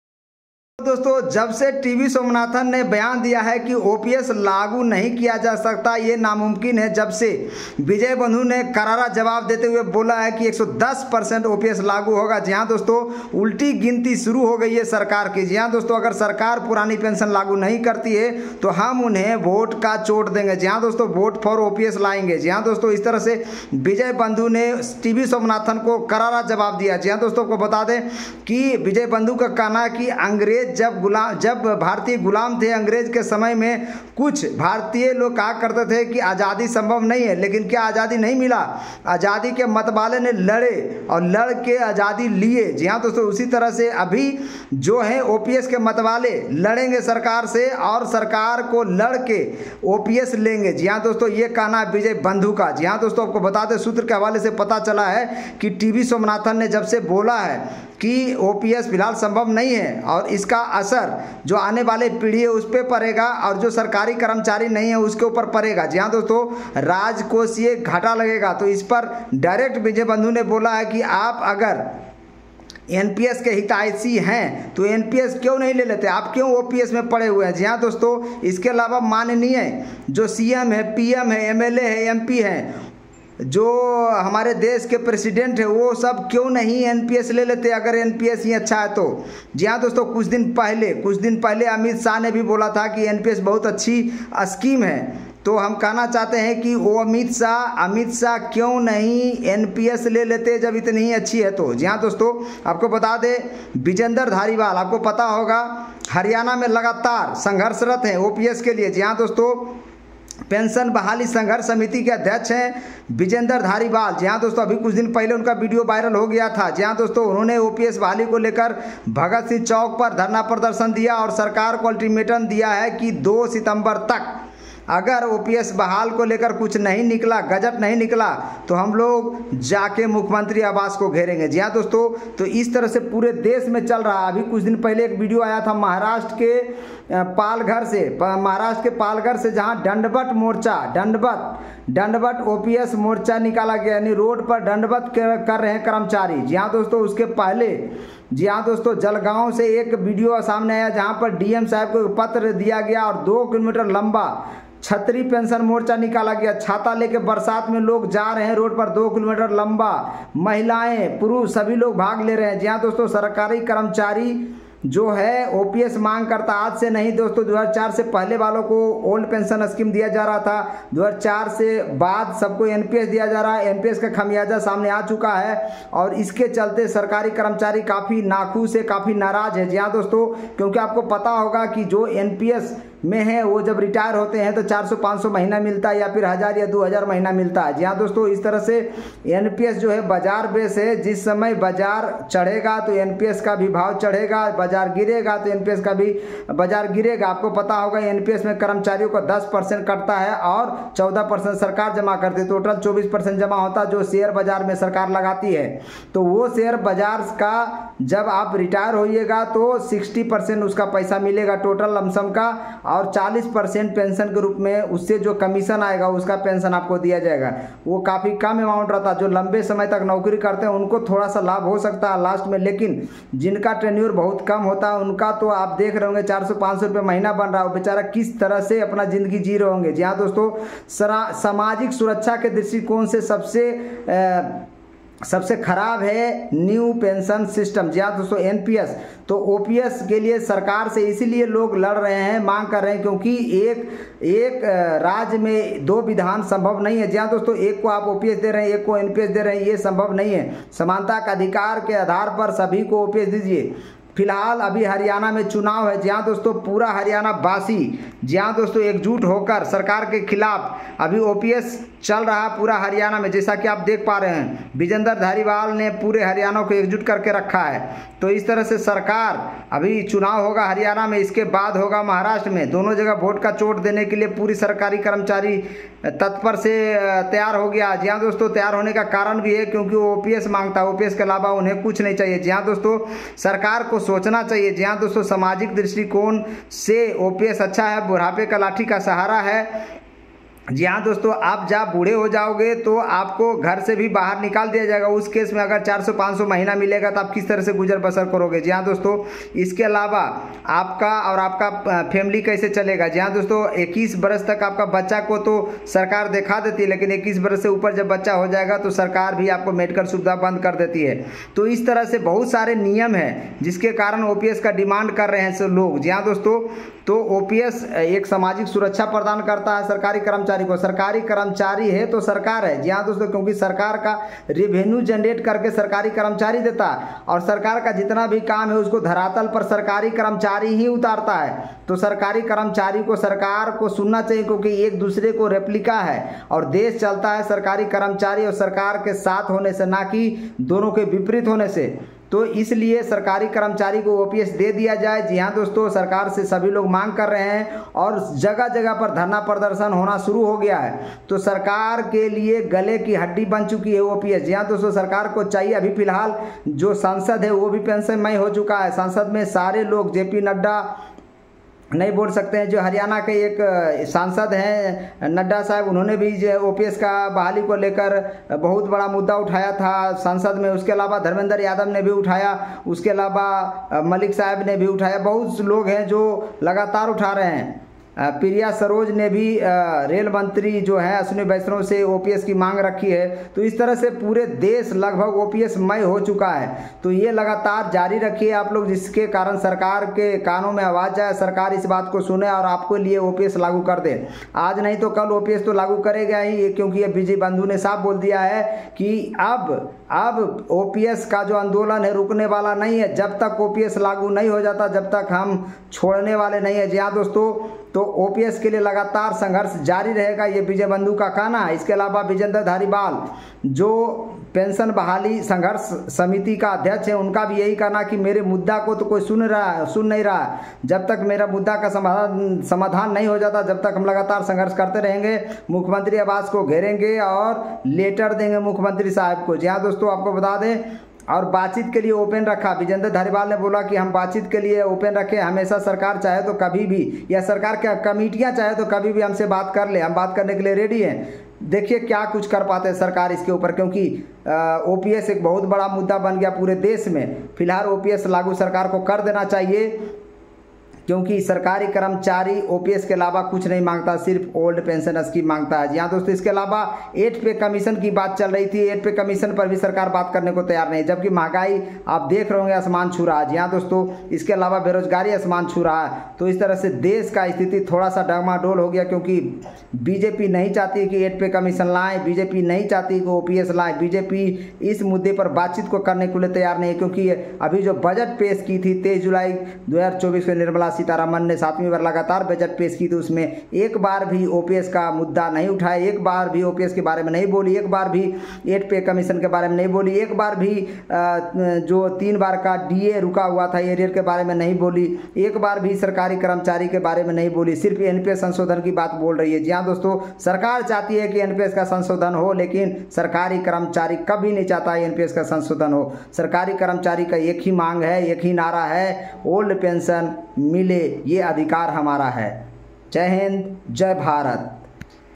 110% मिलेगी पुरानी पैंशन। वोट की चोट देंगे सरकारी कर्मचारी। अब रुकने वाले नहीं। संघर्ष जारी रहेगा
110% मिलेगी पुरानी पैंशन। वोट की चोट देंगे सरकारी कर्मचारी। अब रुकने वाले नहीं। संघर्ष जारी रहेगा